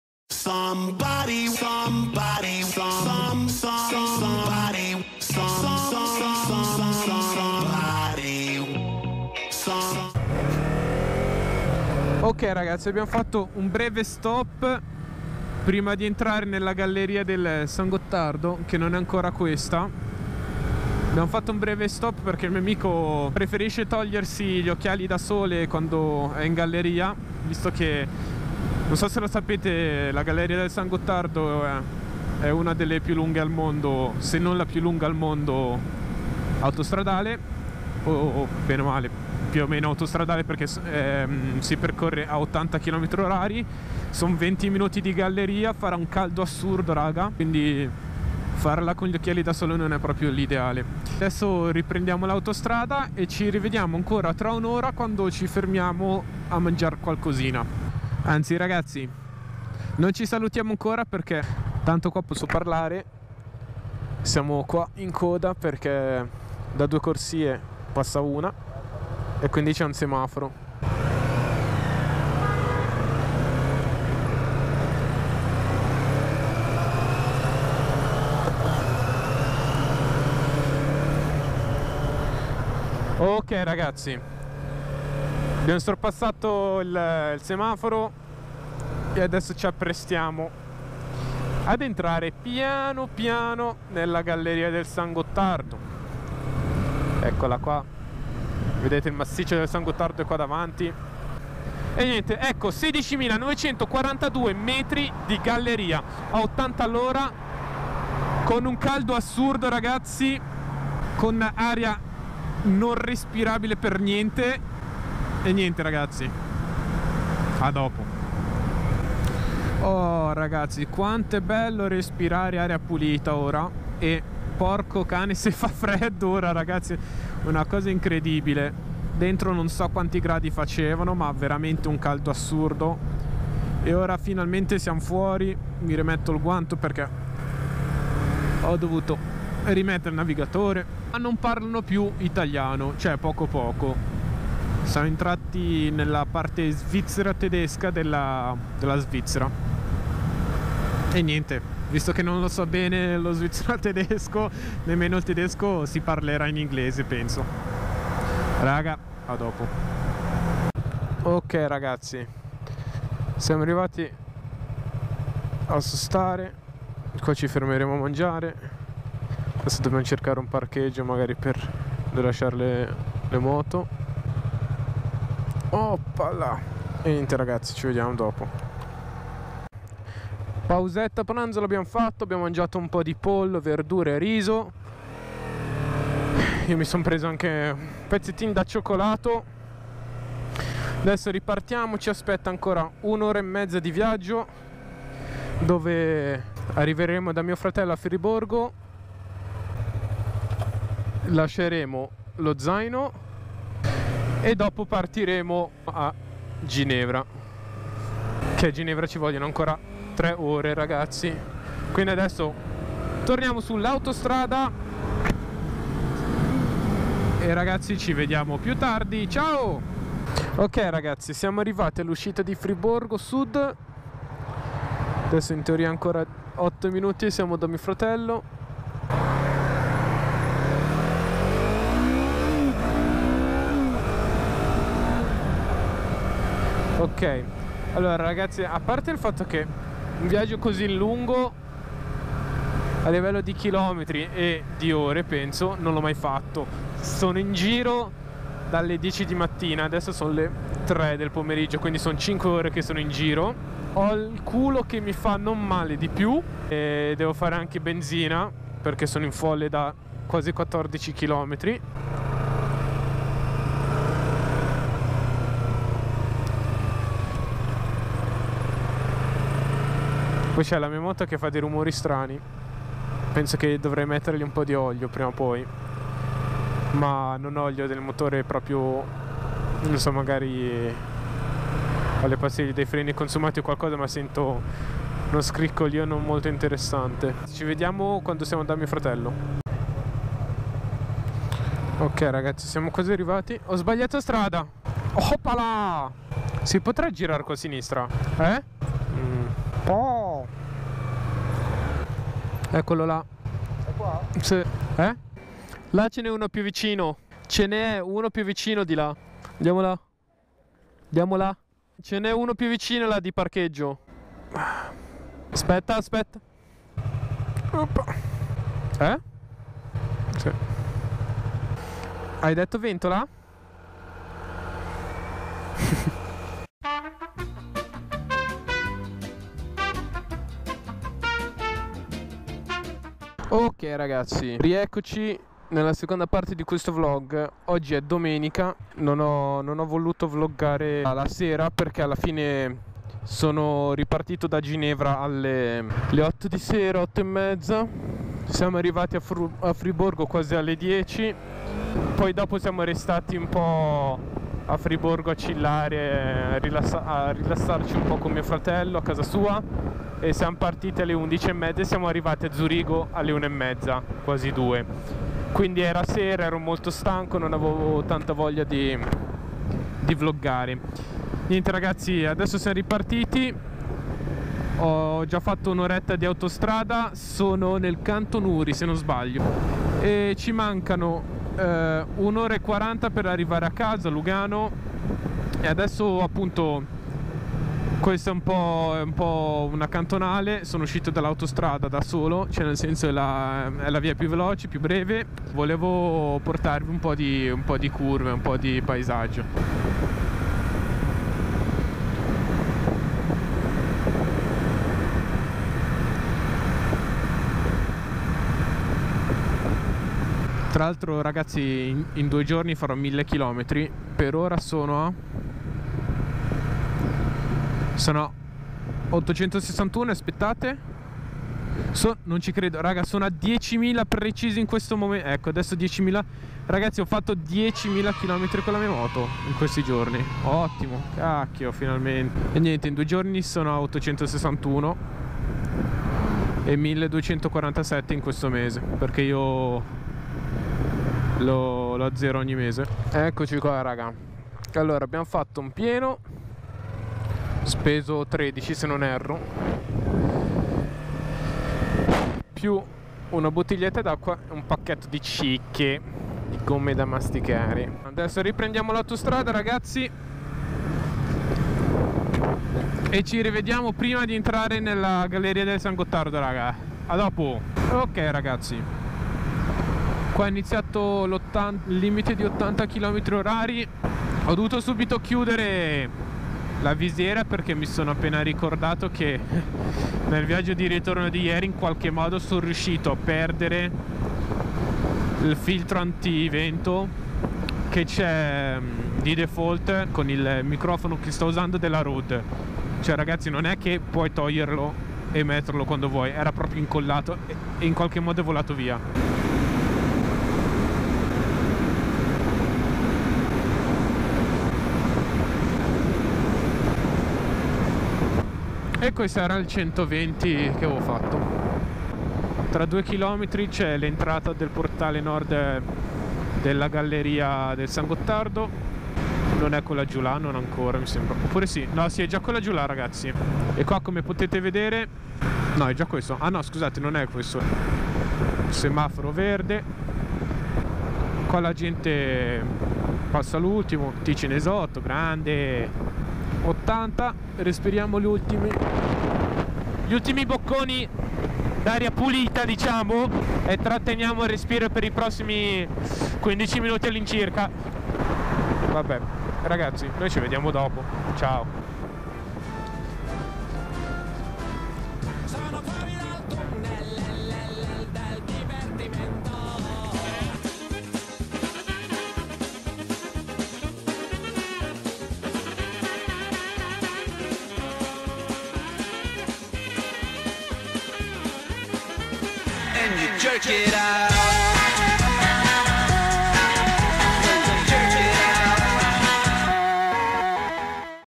1000 KM in due giorni - Lugano-Friborgo-Ginevra--Zurigo-Lugano
Ciao ragazzi,

finalmente sono tornato con un nuovo vlog in sella alla mia Yamaha MT03.
Sedetevi e gustatevi questo video!

Moto:
Yamaha MT03 del 2016
Tutti i pezzi della moto sono originali.